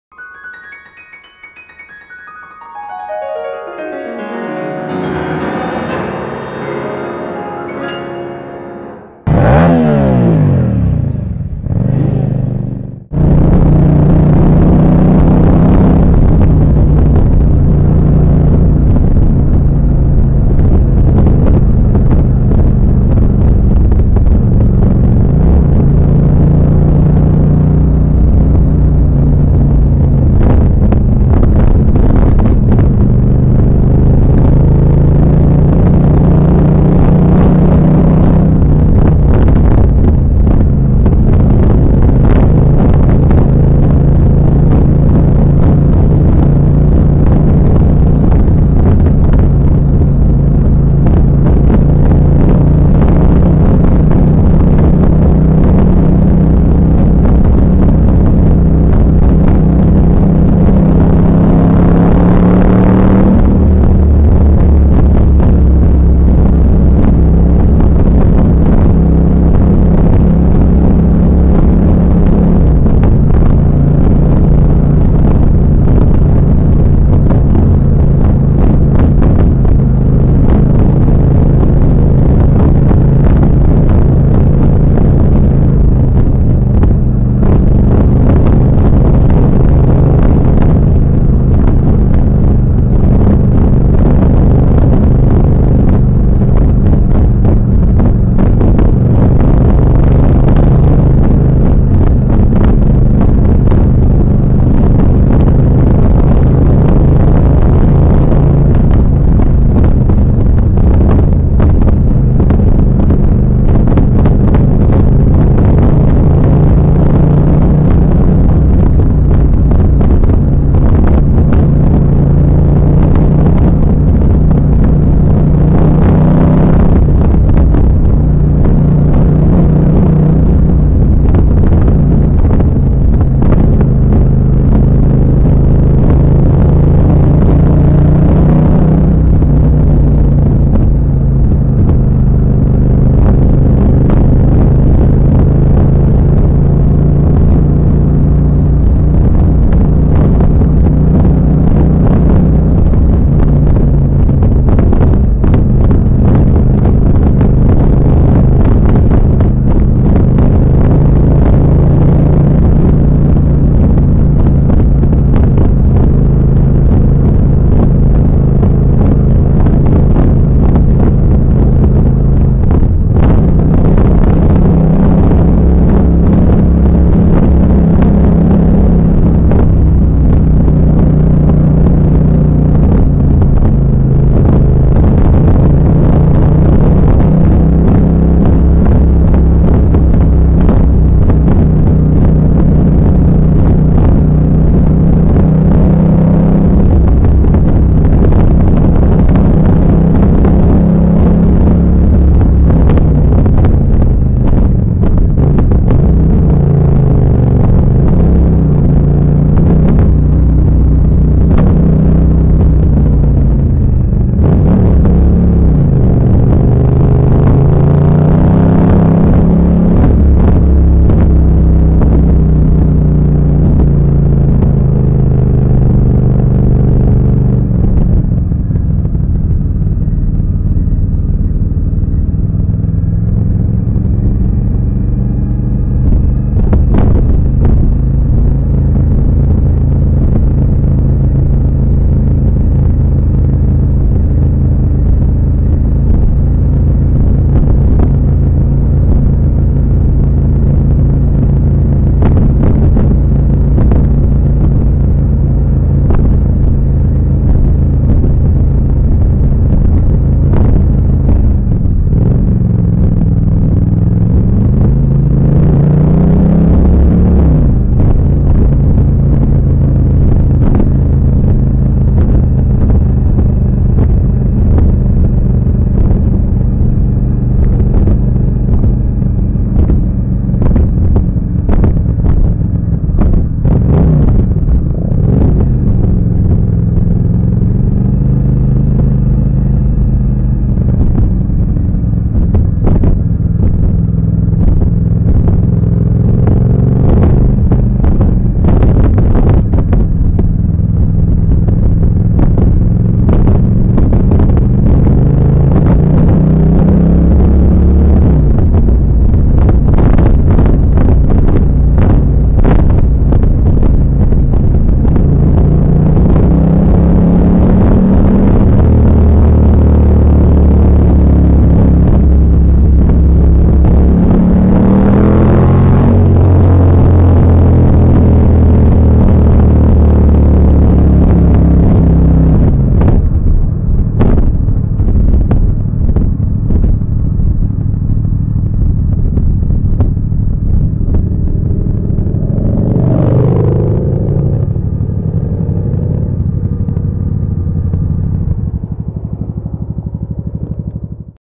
The Peak of Gate's Pass
A cruise through this incredibly scenic location with a subscriber: The 7_12. Enjoy the ride!